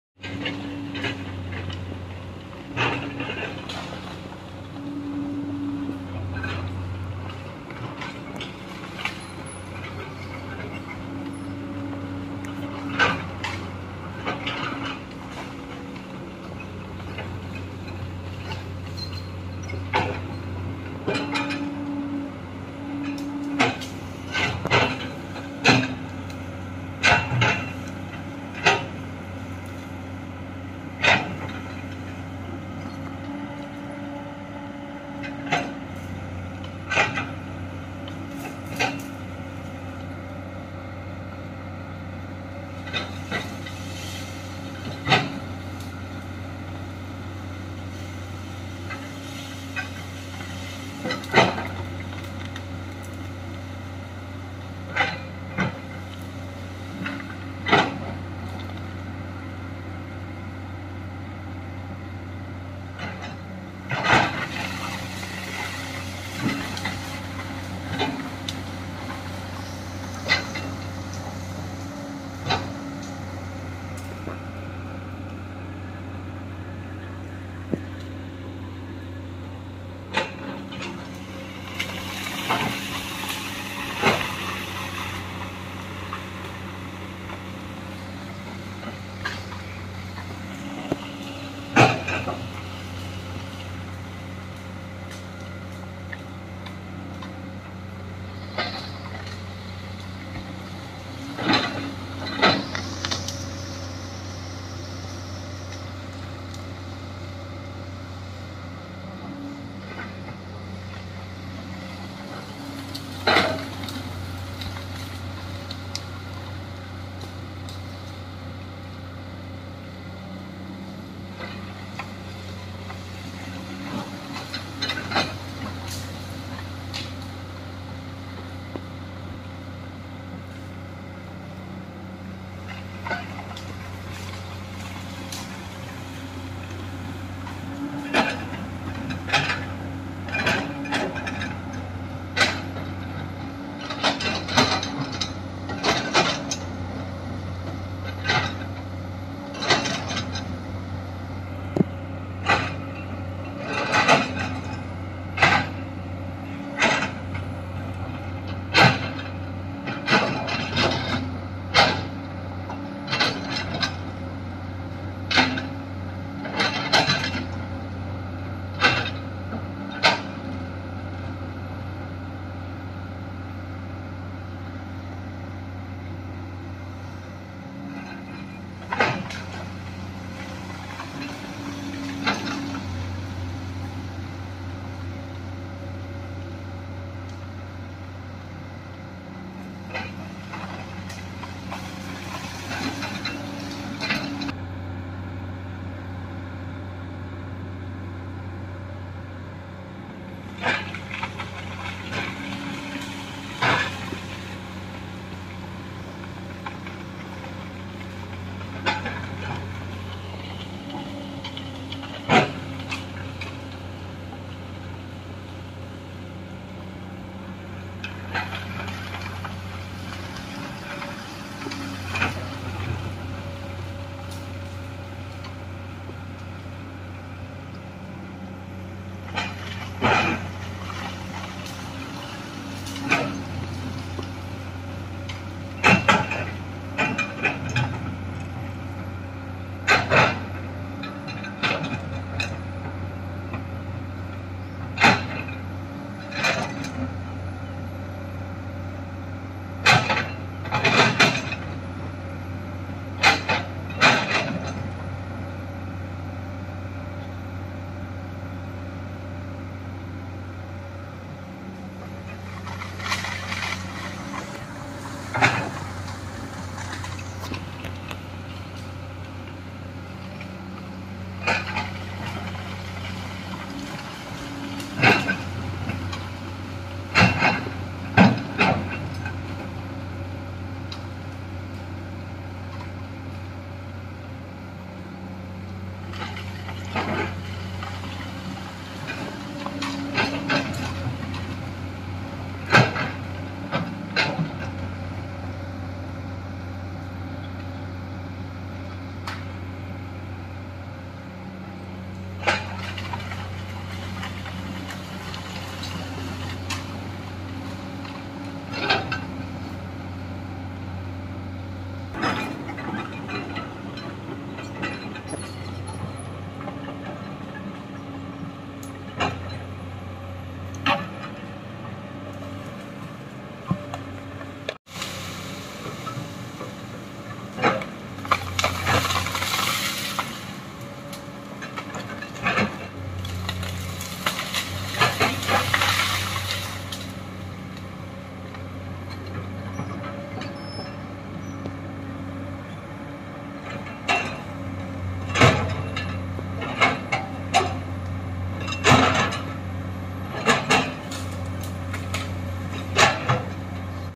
How To Make a New Road l New Road Make With Komatsu Excavator l JCB Video l Excavator l JCB Cartoon
Excavator Operator
From :- Assam

Live :- Mizoram